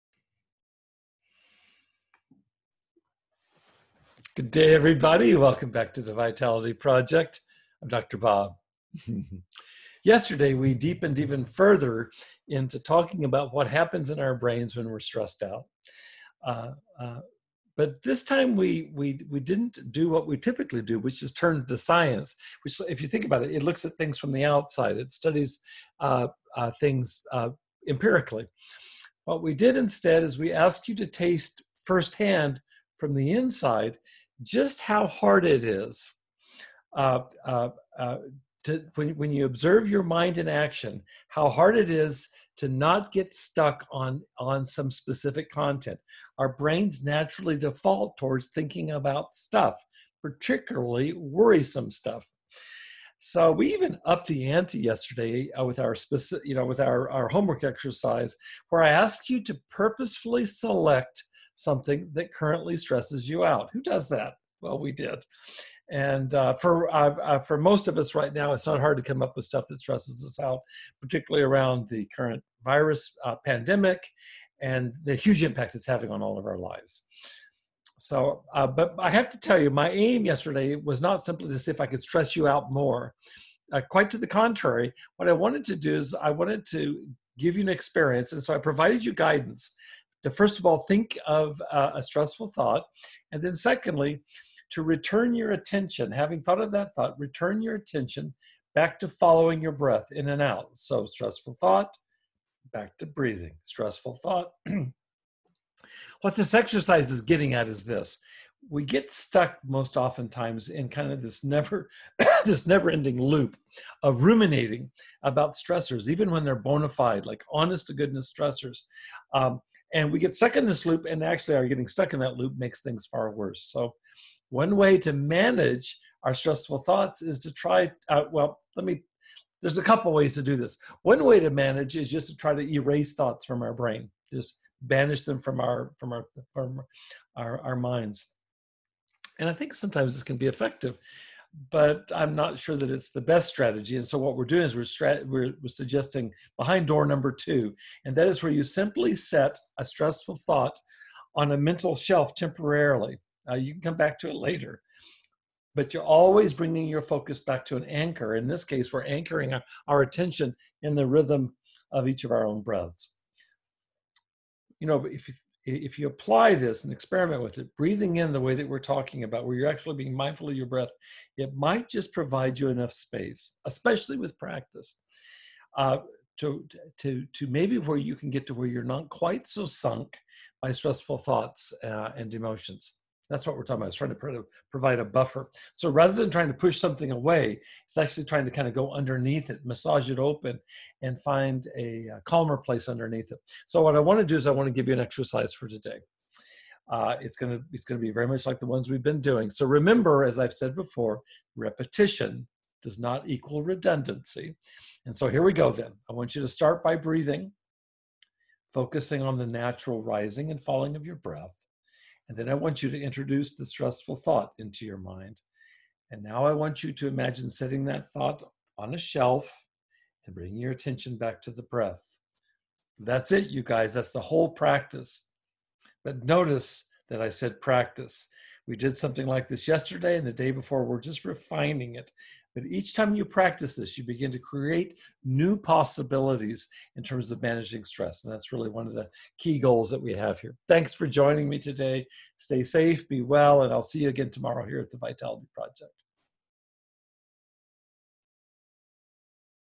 How to Break the Loop of Stressful Thinking | 4/30/20 | Rollin' Out of Bed With Dr. Bob
Good day, and welcome back to the Vitality Project. I'm Dr. Bob!

Yesterday we deepened even further into what happens in our brains when we are stressed. But this time we did not turn first to science, which studies phenomena from the outside. Instead we asked you to taste first-hand, from the inside, just how hard it can be to simply observe your mind in action, without identifying with any of its particular content.

We even upped the ante in our specific homework exercise: requesting you to purposefully select something that currently stresses you out (which, for many of us, centers around the current virus pandemic and its huge impact on all our lives). Our aim, however, was not simply to stress you out more. To the contrary, we provided guidance for you to: first, think the stressful thought; then second, return your primary attention to following your breath in and out.

What this exercise is getting at is that our becoming stuck in a never-ending loop of ruminating about honest-to-goodness stressors most often actually makes them far worse. One way to manage our stressful thoughts is to try to permanently banish them from our minds; and this can at times be effective. But we were suggesting another strategy: simply setting that stressful thought on a mental shelf temporarily (you can come back to it later), always returning your focus back to the rhythm of your own breath. Breathing in mindfully may provide just enough space, especially with practice, to where we may not get quite so sunk as before by stressful thoughts and emotions.

Today’s homework:
I would like you to try that same exercise again today (remember: repetition does not equal redundancy!). Here we go then: start by breathing, focusing on the rising and falling of your natural breath, then introduce the stressful thought. Now gently set that stressful thought on an imaginary shelf in your mind, bringing your attention once again back to your breath. That’s it, the whole practice! But notice that I said “practice”…